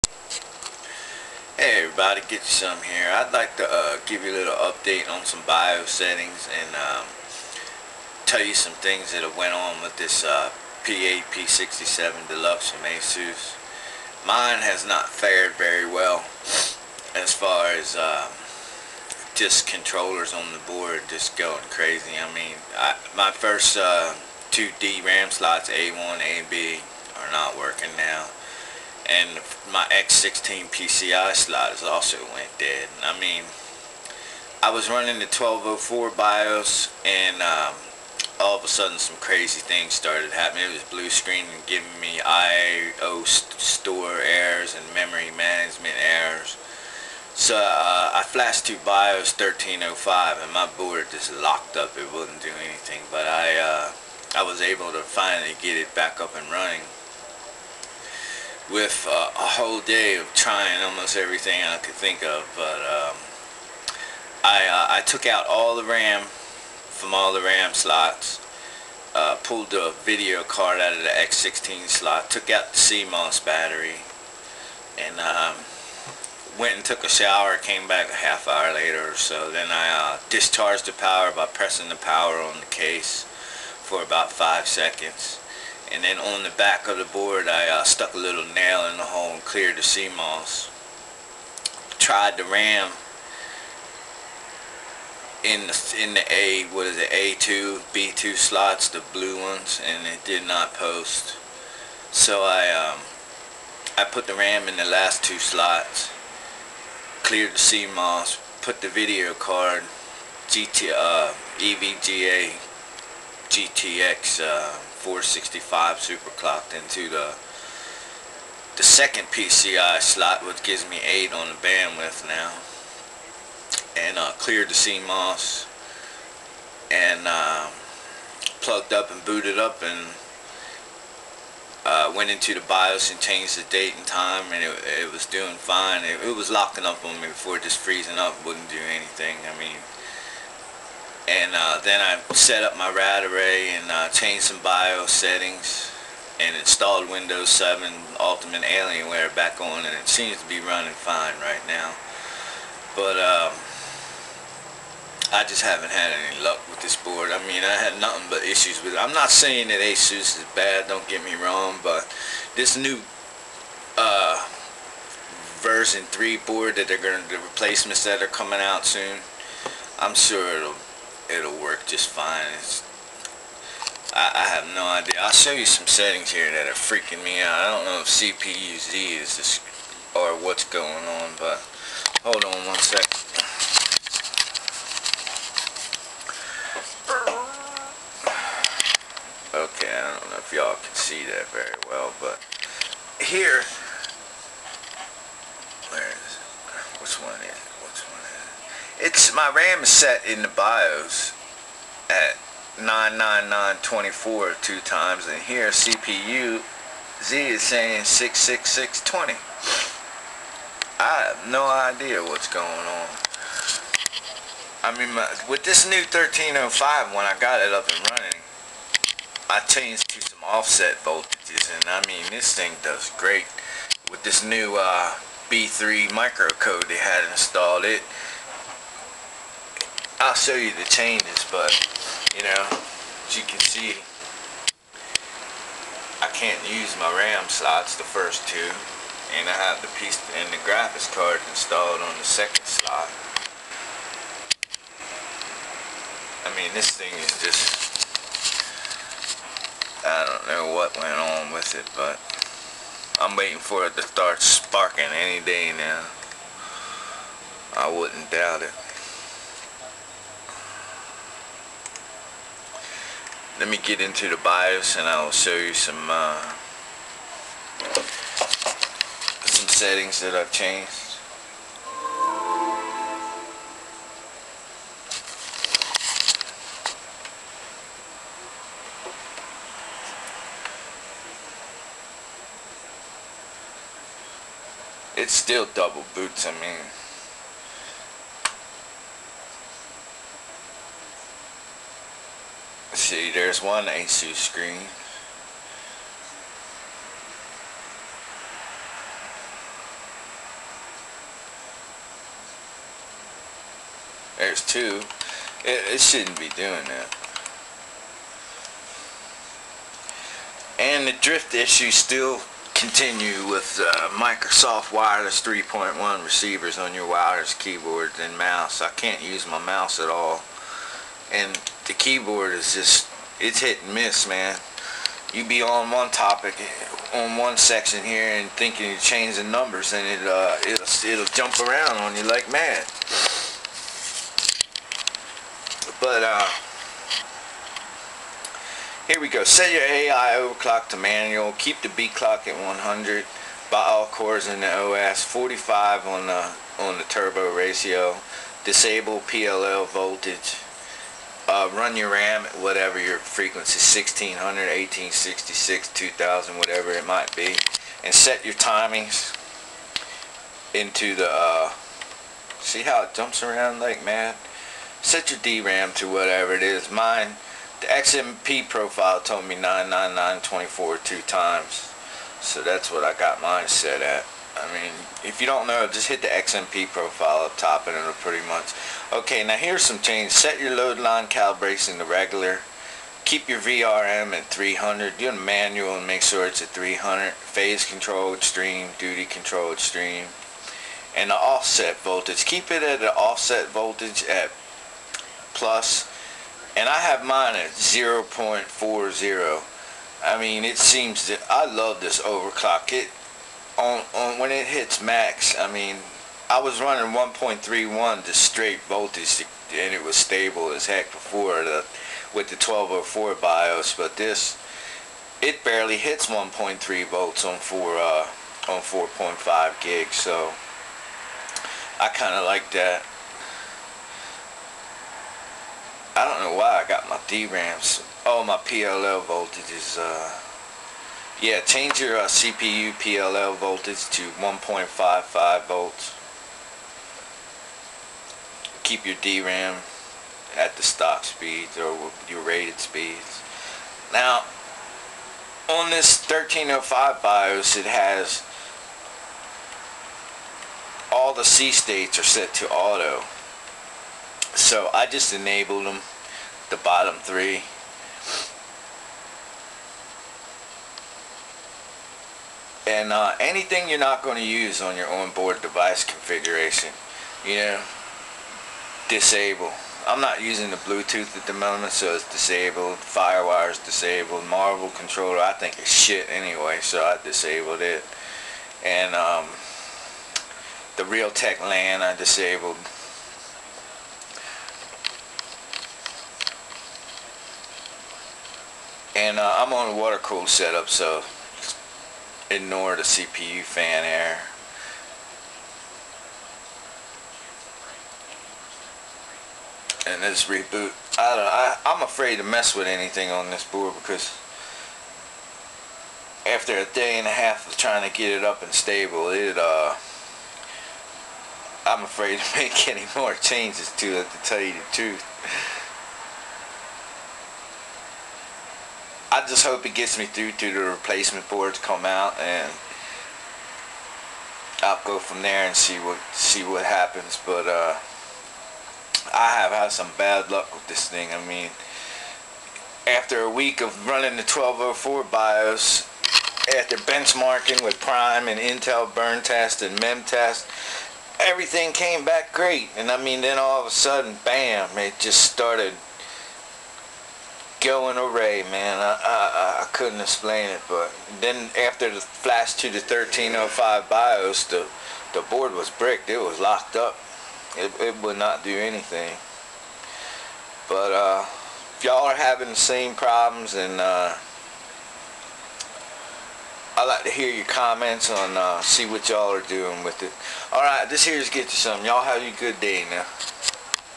Asus P8P67 Deluxe 1305 Bios
0.040 on the offset as I said in the video 0.40 , SORRY Mates..I do not advise anyone to update to the 1305 Alpha/Beta Bios , Asus will have a final version of this Bios in the download section in a few days or so. When it gets released the settings in this video should yield some Outstanding Overclocking results. 0.040 on the offset as I said in the video 0.40 , SORRY Mates.

For everyone who now has a Bricked board I do understand your Frustration.